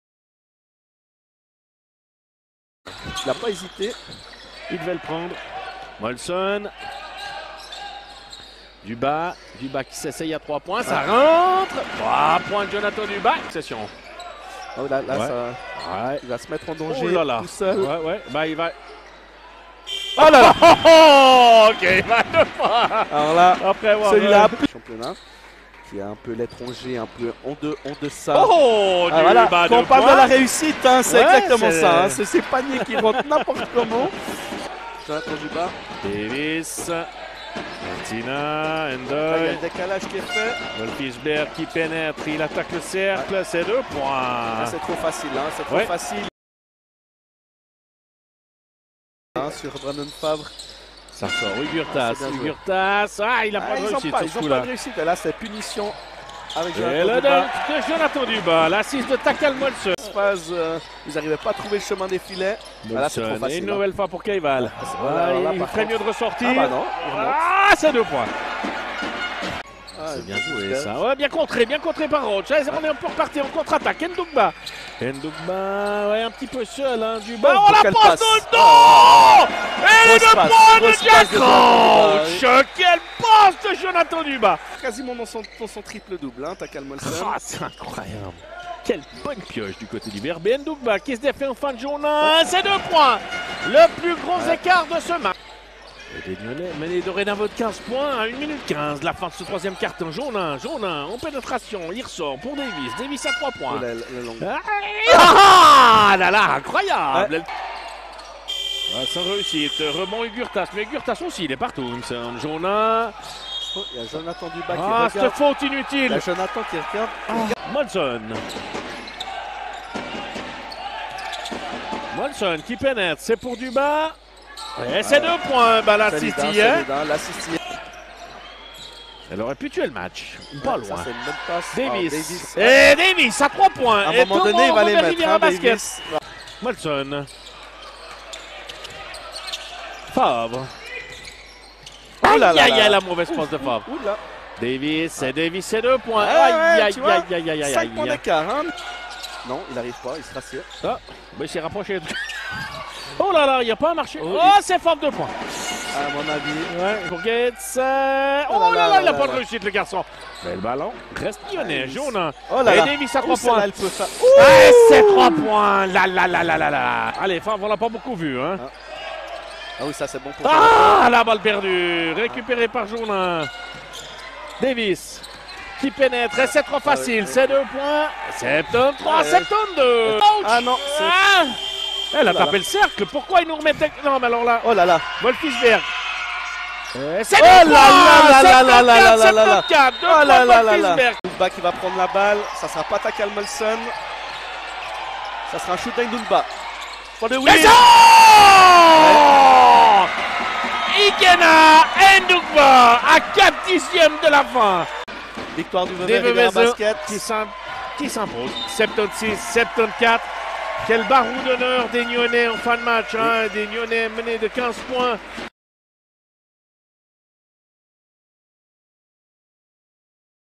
Vevey Riviera Basket vs. BBC Nyon - Game Highlights
Watch the Game Highlights from Vevey Riviera Basket vs. BBC Nyon, 11/04/2023